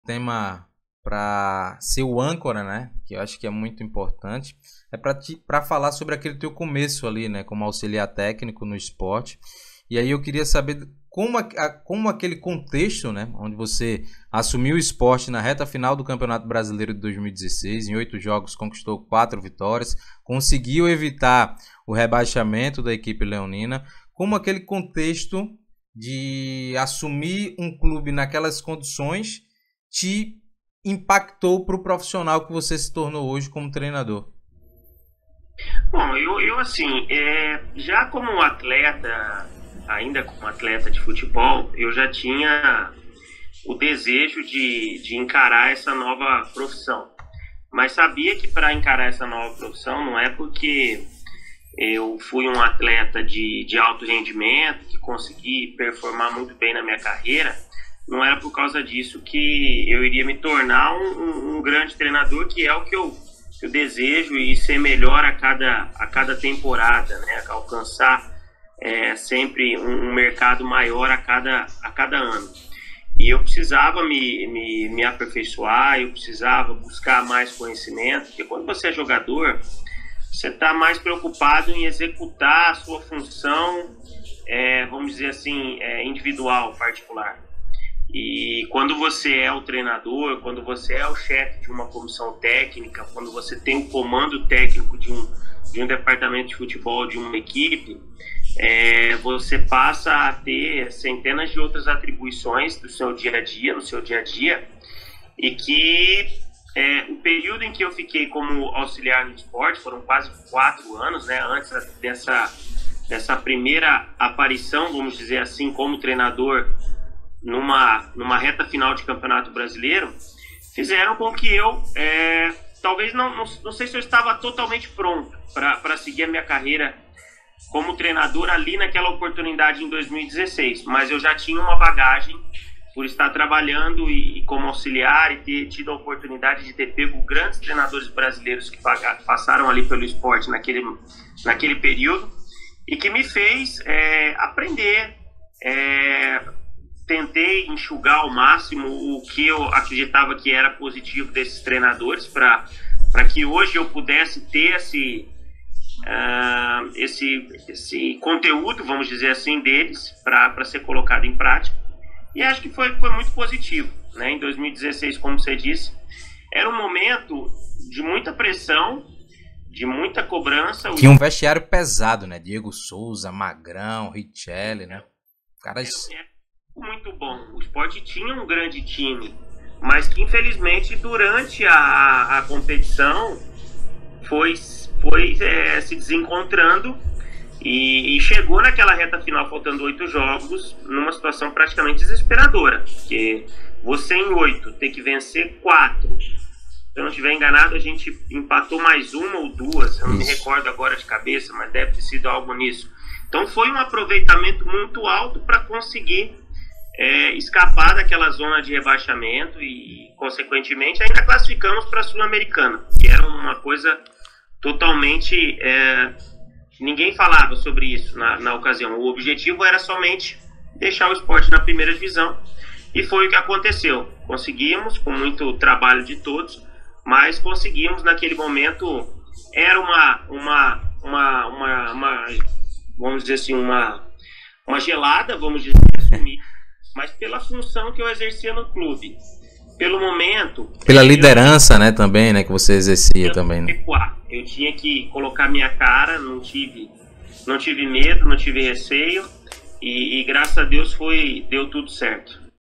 DANIEL PAULISTA FALA SOBRE OS BASTIDORES DO SPORT EM 2016 QUANDO EVITOU O REBAIXAMENTO
O Campo Fala #9 recebeu o técnico Daniel Paulista

As análises táticas das equipes e jogadores nordestinos são produzidos com auxílio do KlipDraw Motion. Para mais informações acesse